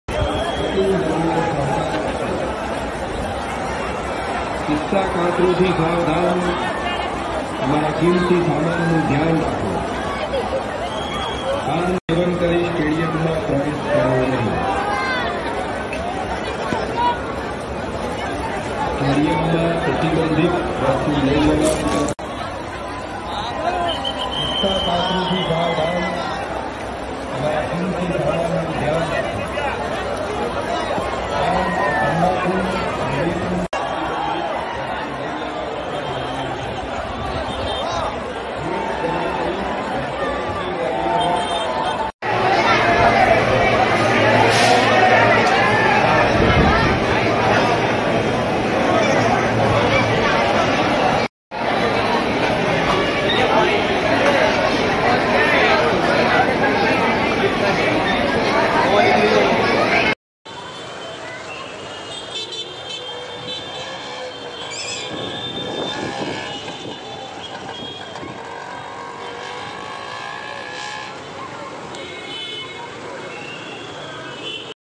IPL: अहमदाबाद के नरेंद्र मोदी स्टेडियम के बाहर का माहौल | GT vs MI Qualifier 2
Qualifier-2 is scheduled to be played on Friday and the final on Sunday at the Narendra Modi Stadium in Ahmedabad. The fans are very excited about these two matches. The alarm was that there was a stampede at the ticket counter on Thursday for tickets. A crowd of thousands was present outside the stadium amidst the scorching heat.